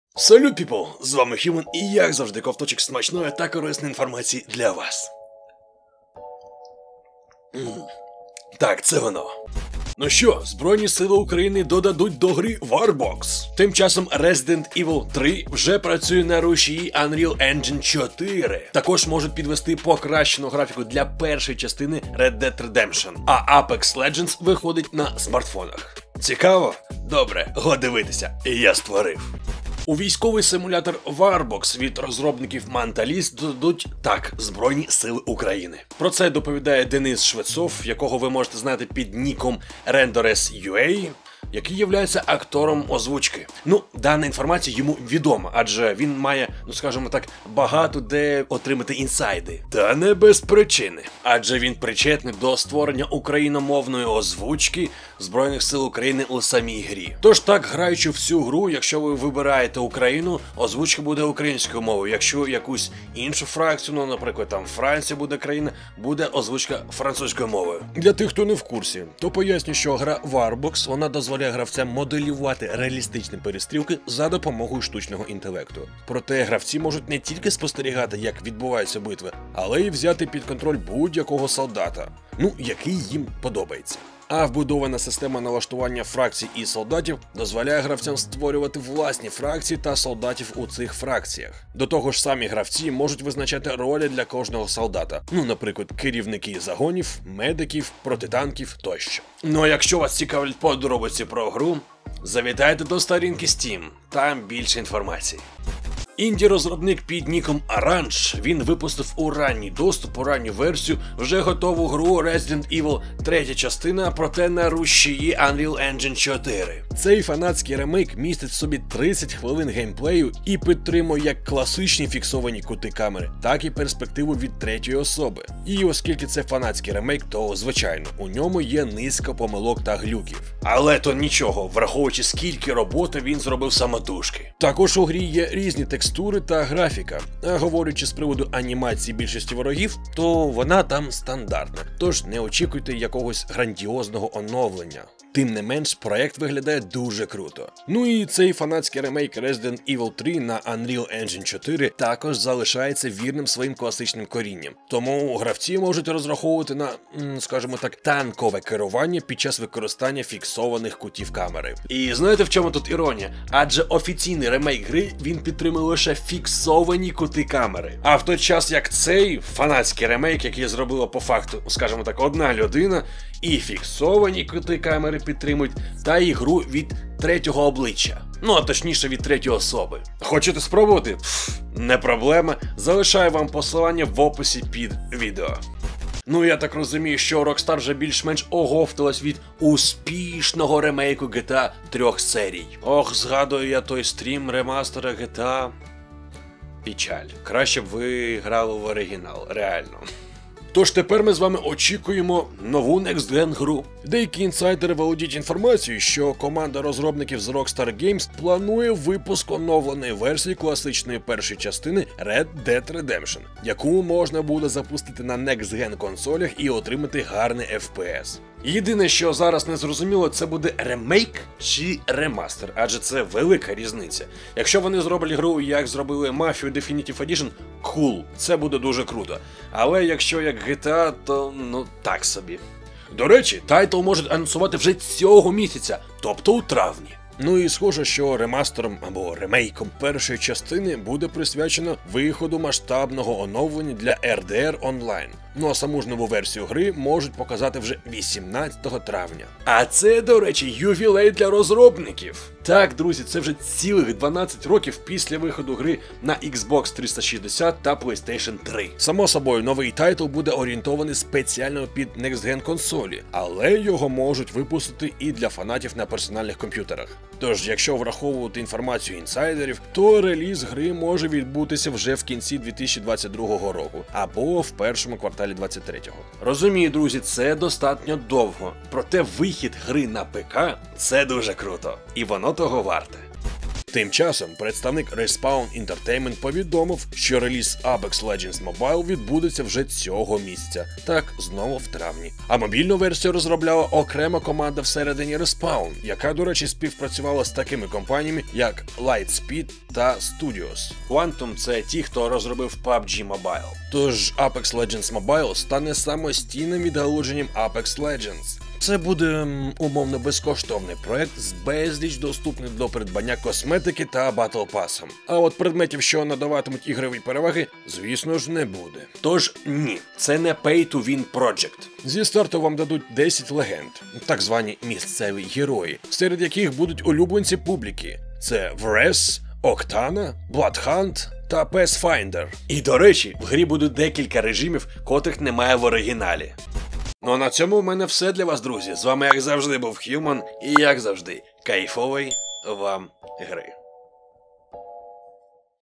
ЗСУ у грі Warbox. Resident Evil 3 на Unreal Engine 4. Оновлений RDR на ПК. Apex Legends Mobile
Збройні сили України додадуть до гри Warbox.
Resident Evil 3 вже працює на Unreal Engine 4.
Можуть підвести покращену графіку до 1ї частини RDR.
А Apex Legends виходить на смартфони.
Го дивитися, я створив!

00:00 салют, піпл! З вами HUMAN!
00:09 анонс відео
00:29 у Warbox додадуть ЗСУ з українською озвучкою
01:46 фанатський ремейк Resident Evil 3 на Unreal Engine 4
03:06 анонс нового некстген Red Dead Redemption
04:43 реліз Apex Legends Mobile у травні
05:36 дякую, друзі!

💳Крипто-гаманець (eth, bnb, busd, usdt, ltc, matic): 0x9Bee954427E7bA8d20e0f276BcAB22Dc5056b394
💳Крипто-гаманець 2 (USDT/Tron trc 20): TMwqaiubG3uNxa4dkSh4EpSepYcbpkwSxo